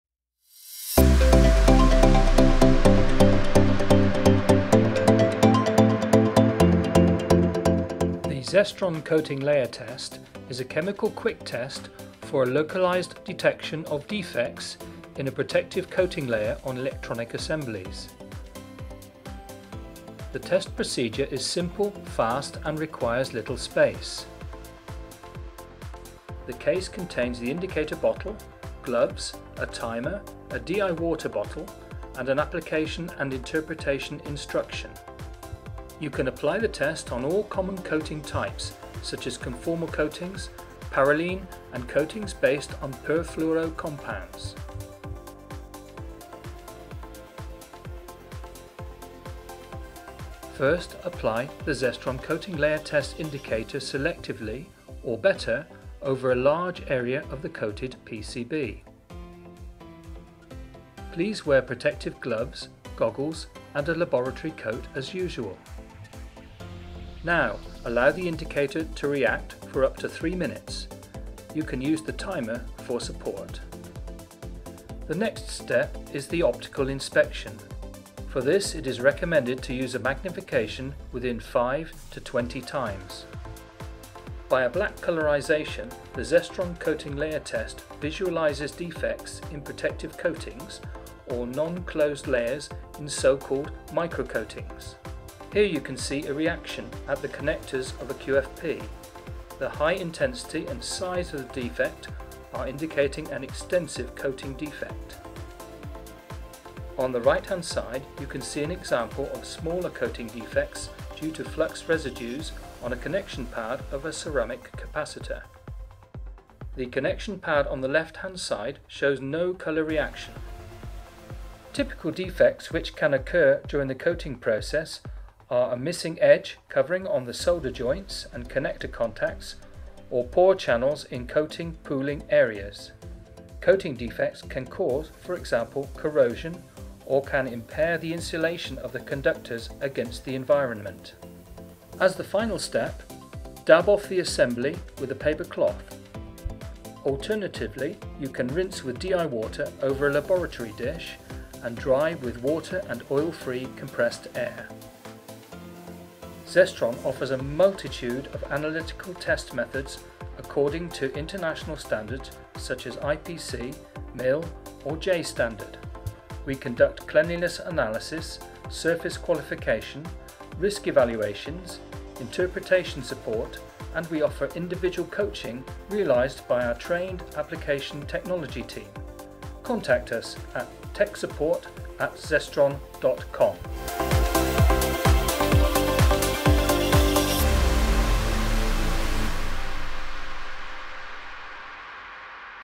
Detection of protective coating layer defects on electronic assemblies
The ZESTRON® Coating Layer Test utilizes a black color reaction as a visual indicator of the defects in the protective coating, even in the case of μ-coatings. The test thus adds the standardized methods for coating thickness measurement by enabling rapid and non-destructive detection of closed and dense coatings. Therefore, the test can also be used during production for cost-effective sampling.
The reliability of protective PCB assembly coatings, with regard to climate and harmful gas safety, is essentially determined by the uniform application of the conformal coating without interferences or gaps in the protective layer. In particular, common trouble areas such as solder joint edges and pore channels in coating pooling areas, when lacking a proper coating layer, have detrimental effects on the final board assembly.